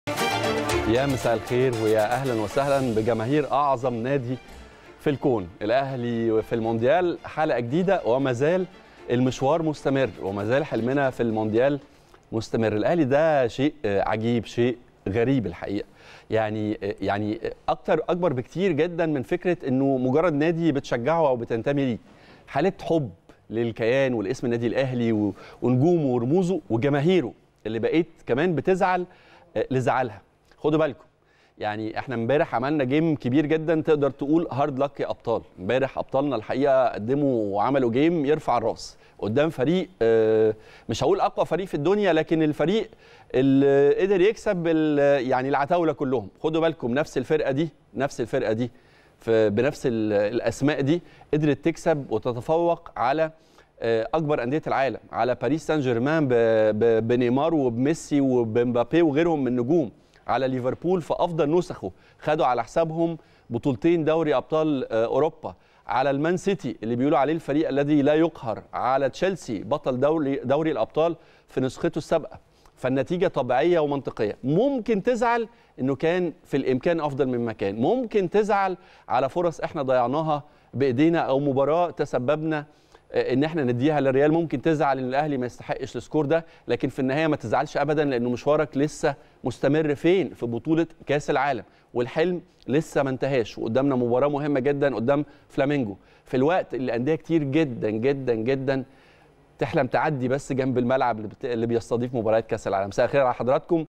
أحمد سمير: الأهلي أكبر من فكرة أنه نادي بتشجعه.. أبطالنا قدموا مباراة رائعة أمام أقوى فريق في العالم
أحمد سمير: الأهلي أكبر من فكرة أنه نادي بتشجعه.. أبطالنا قدموا مباراة رائعة أمام أقوى فريق في العالم "ممكن تزعل إن كان في الإمكان أفضل مما كان" بس متزعلش لأن المشوار والحلم لسه منتهاش